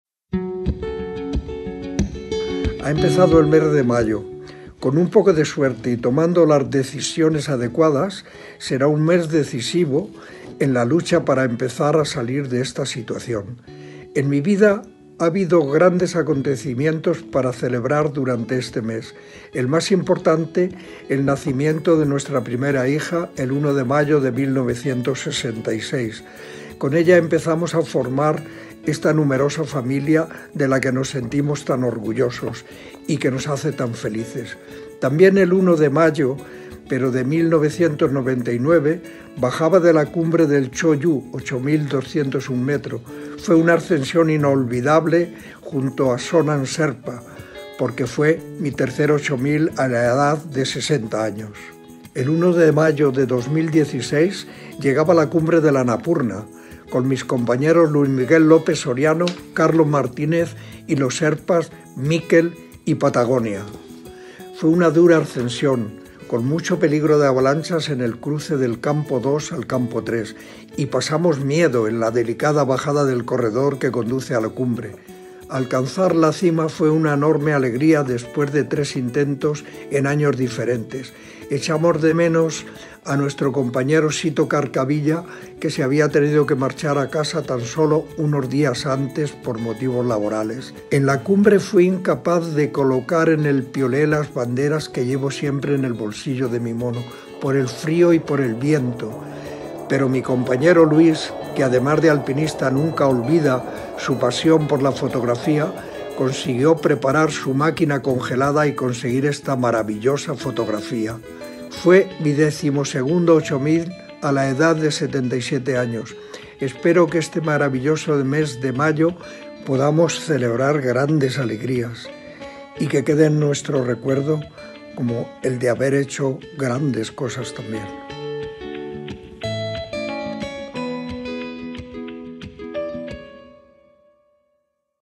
Un 1 de mayo para recordar
El 1 de mayo es una fecha muy especial en mi vida. Siempre he tenido acontecimientos muy importantes para celebrar.

El 1 de mayo de 1966 nacía nuestra primera hija, con la que comenzamos a formar una gran familia, de la que nos sentimos muy orgullosos.

El 1 de mayo de 1999 alcancé la cumbre del Cho Oyu, mi tercer ocho mil, a la edad de 60 años, acompañado de Sonam sherpa.

El 1 de mayo de 2015 junto con mis compañeros: Luis M L Soriano , Carlosmartinezdoc Mikel Sherpa y Himalayan Sherpa pisaba la cima del Annapurna, después de tres expediciones.

Este maravilloso mes de mayo espero  tener motivos para seguir celebrando grandes alegrías y que nos quede en el recuerdo como aquel en el que hicimos grandes cosas.

¡Un abrazo muy fuerte y mucho ánimo para todos!!!